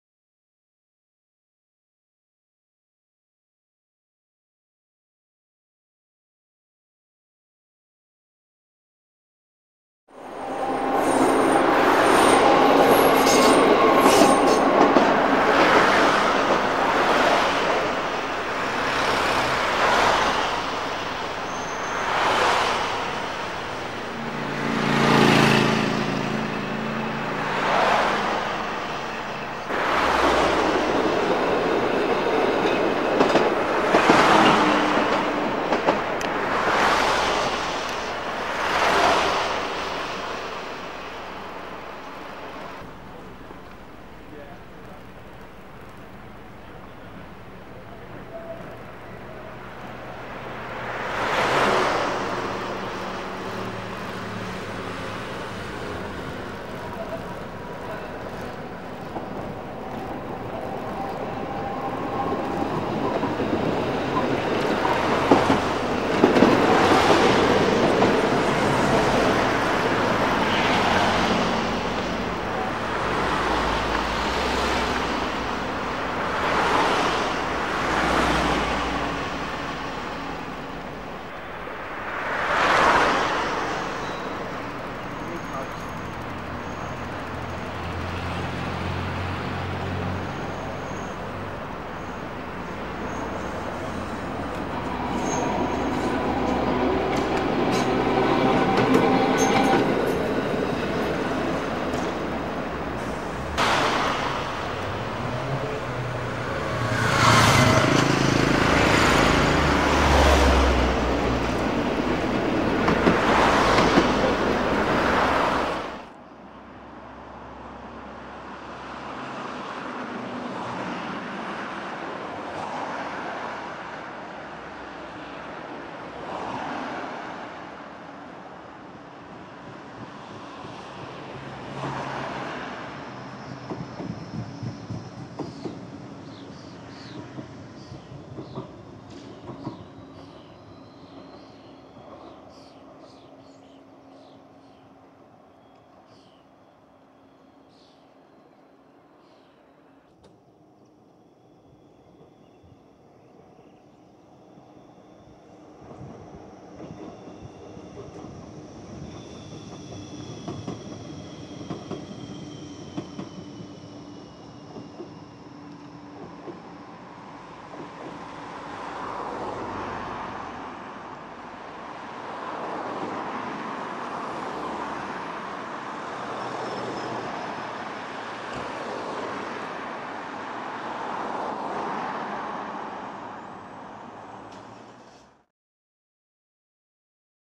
Orion Curve Cleveleys (Blackpool Tramway) Contrasts
The same location on the Blackpool - Fleetwood Tramway as seen in 1991 and 2012... just different trams!.

The 1991 footage was filmed using a tripod and a high quality external microphone. The 2012 footage was filmed in 16:9 widescreen but I edited it to 4:3 format to match the 1991 footage.